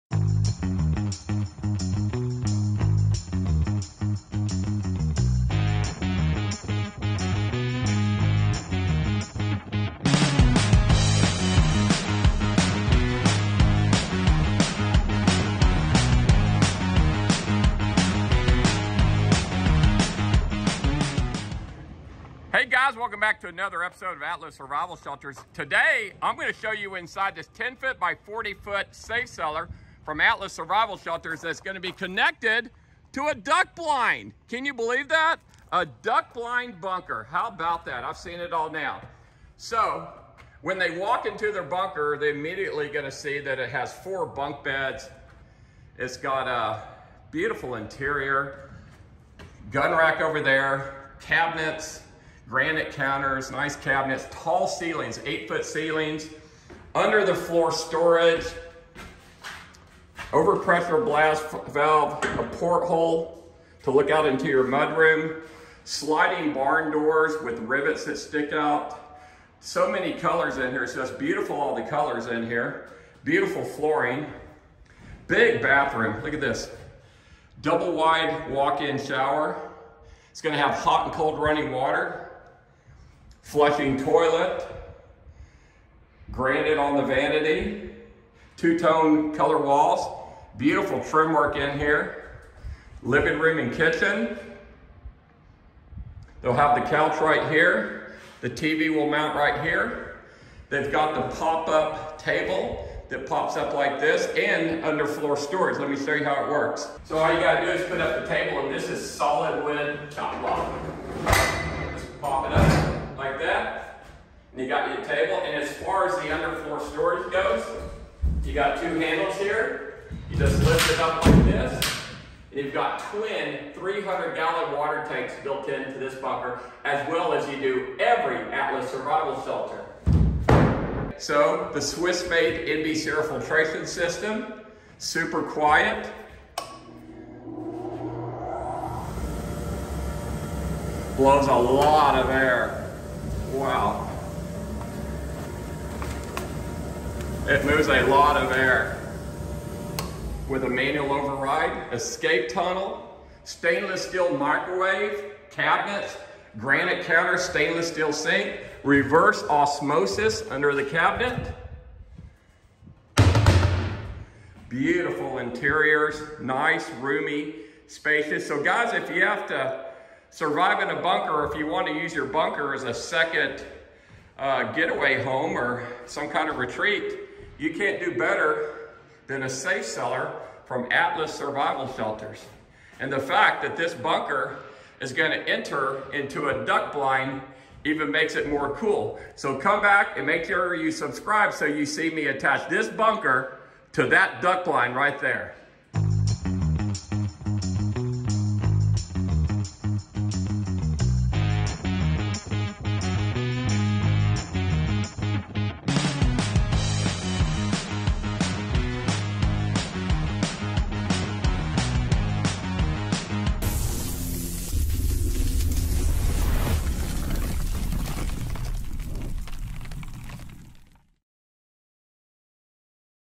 ATLAS Safe Cellar 10x40 Platinum Series Custom Interior
This custom Duck Blind Bunker is a 10x40 Safe Cellar in the Platinum Series
made by ATLAS Survival Shelters. This custom bunker is nice enough to live in on those weekends you wish to go hunting.

Ron Hubbard
President/CEO & Founder